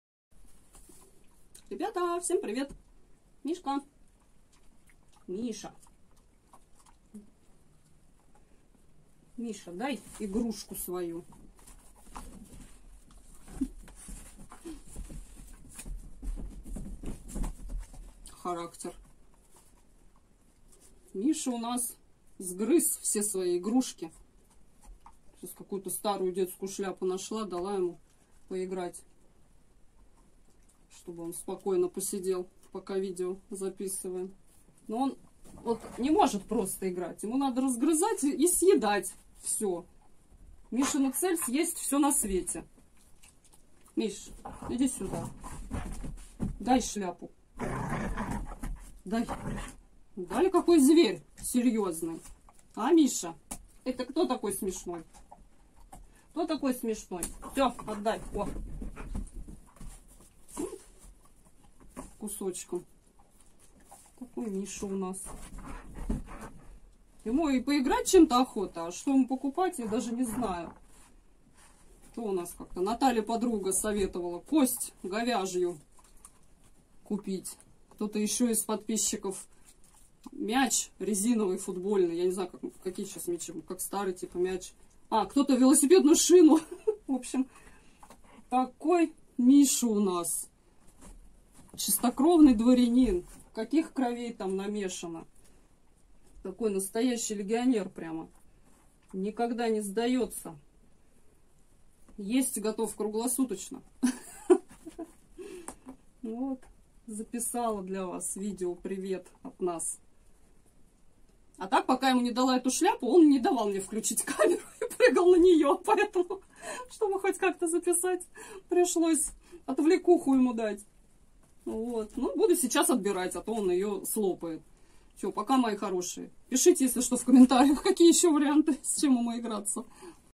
Всё дело в шляпе 😀 Видео привет от Мишки 👋
Видео привет от Мишки! Дело в шляпе! Игрушки для собак. Всё грызет!